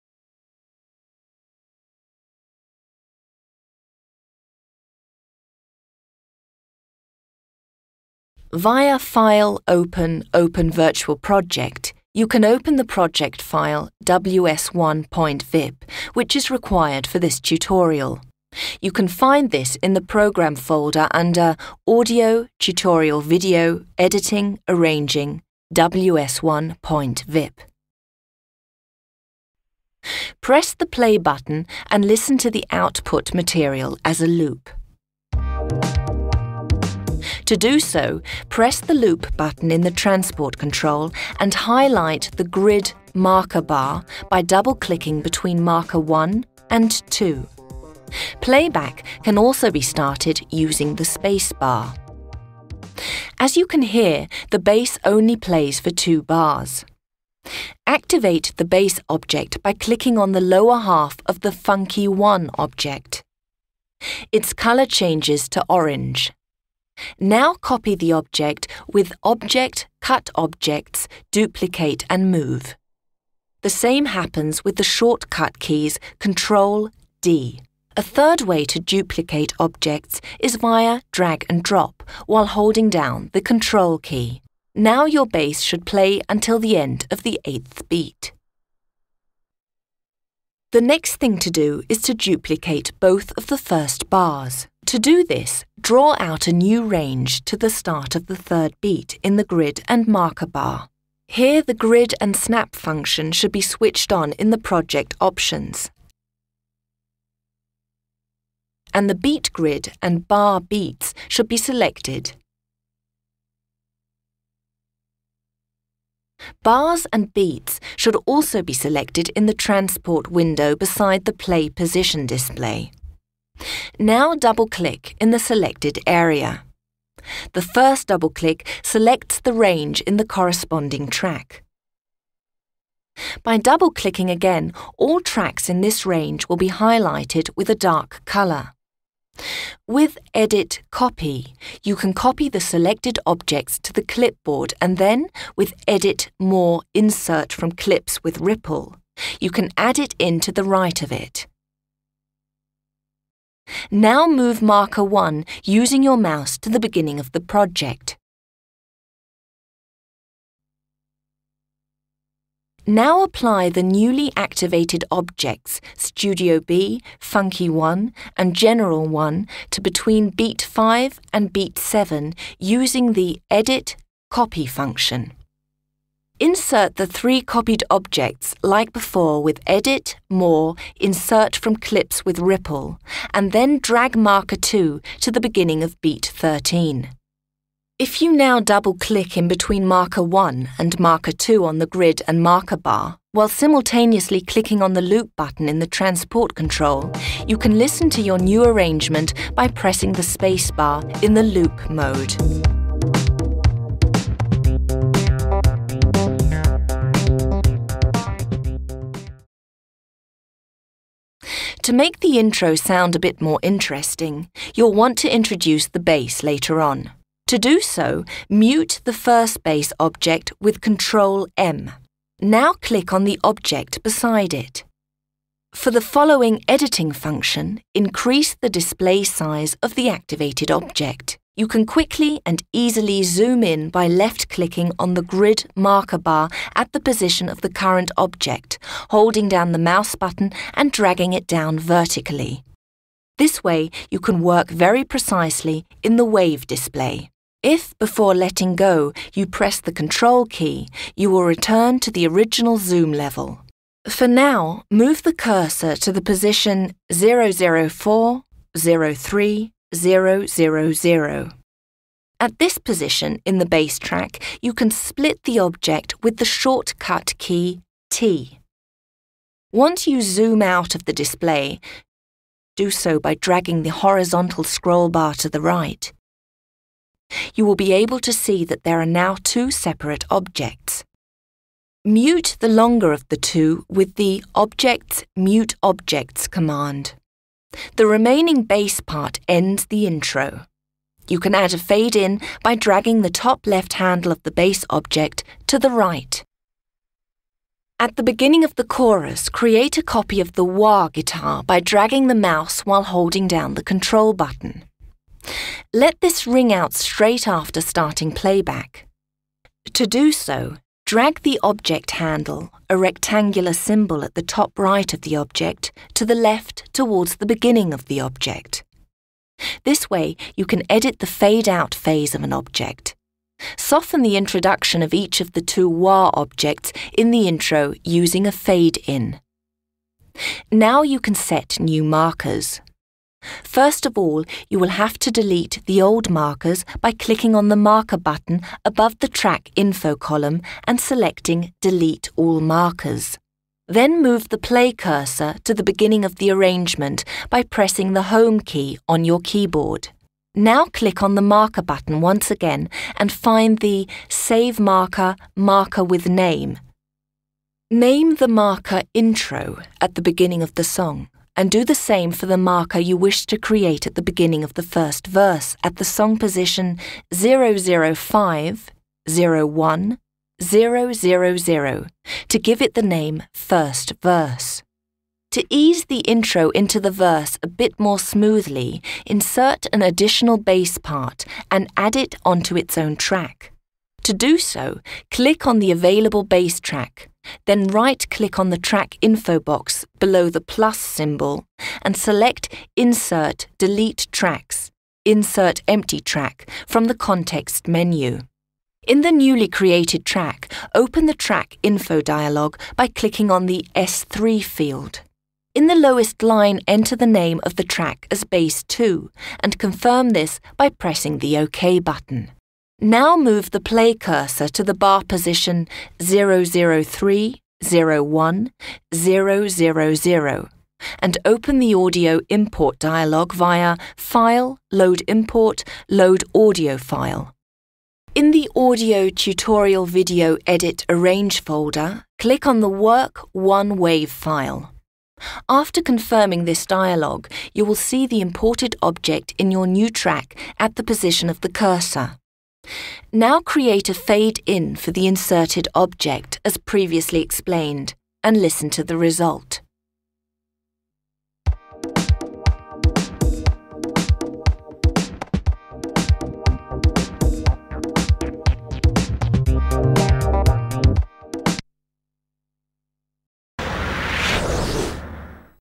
Samplitude 9 / Sequoia 9: Edit and Arrange Part 1
Getting started with Samplitude 9 / Sequoia 9. Here's Part 1 of the Beginner's Guide Tutorial..